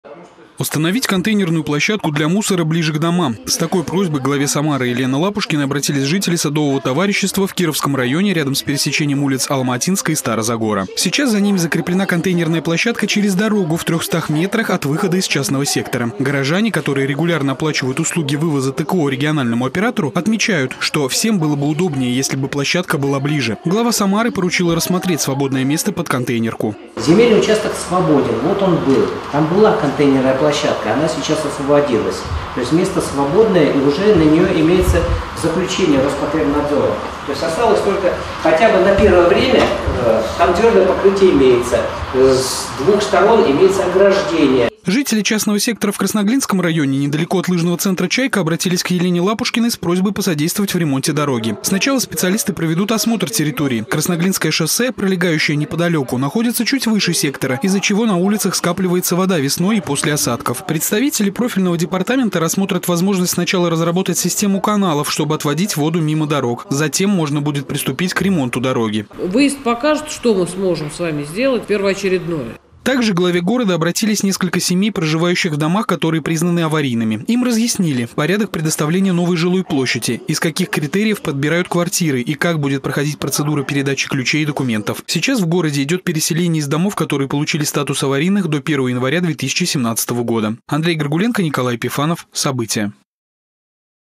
Глава Самары Елена Лапушкина провела личный прием граждан
Глава Самары Елена Лапушкина провела личный приём граждан. Самарцы обратились с вопросами переселения из аварийного жилья, ремонта дорог и установки контейнерной площадки. Какие решения были озвучены, расскажет Андрей Горгуленко.